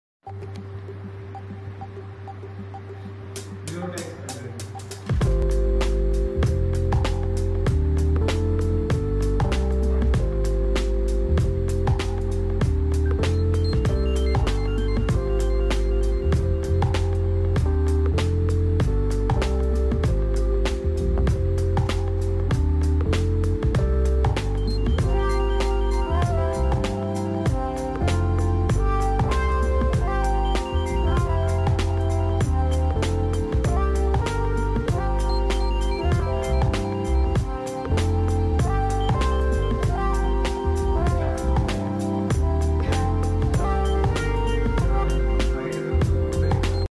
Countdown Machine (Digital Clock)
Countdown machine that can work as a digital clock, designed with accuracy of a shift of time of around 1min per year at max. The countdown time is user settable and the machine uses RTC module to keep time